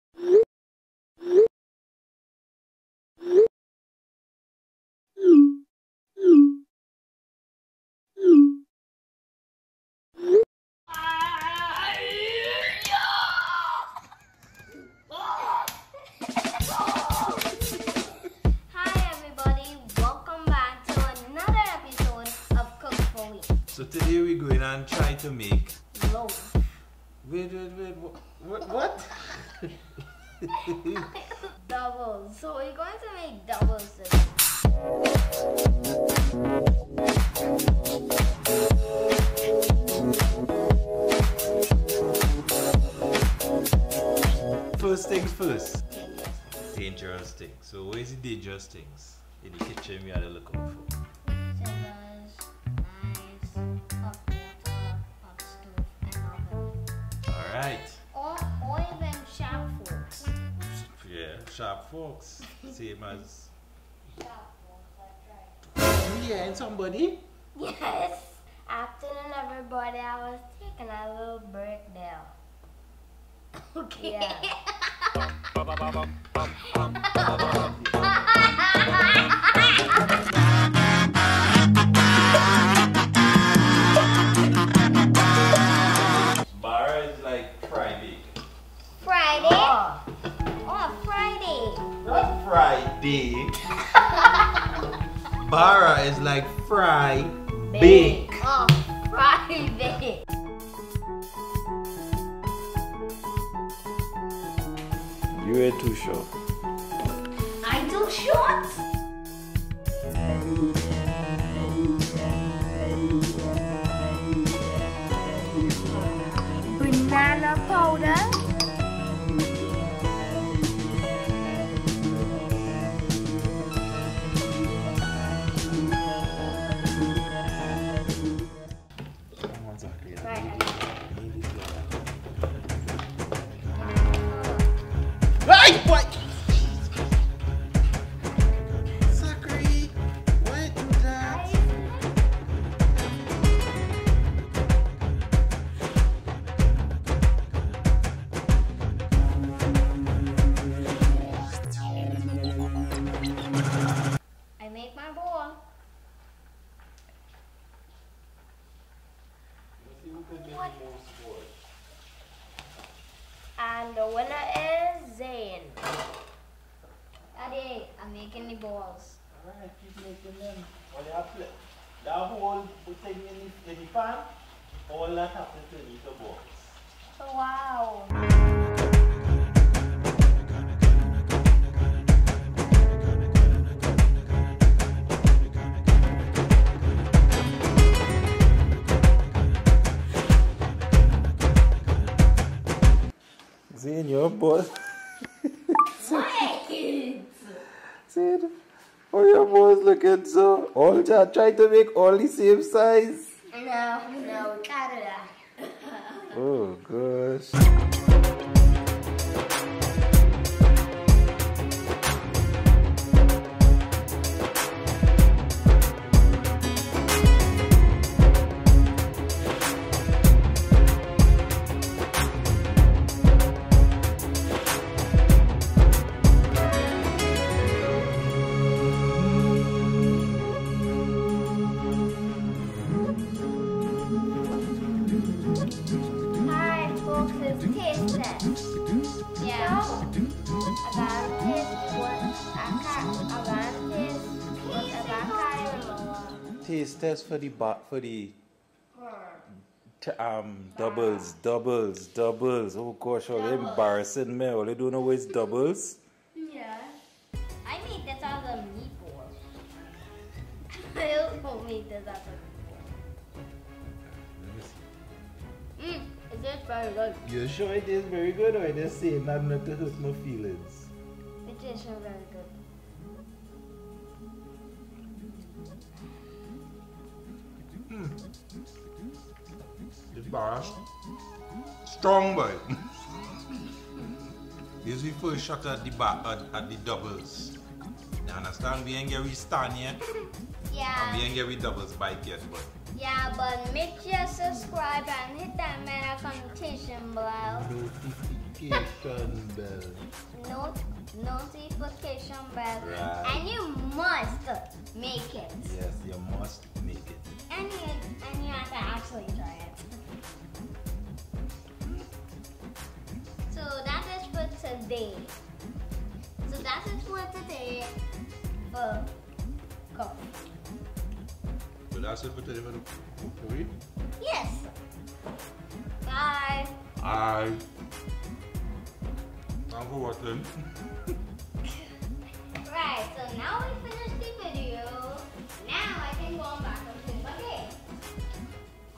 How To Make Trini Doubles? | Cook4we Style
So we asked the question, how to make Trini Doubles? Well lets just say we wont be selling on the stand no time soon. We have alot more work to do...lol.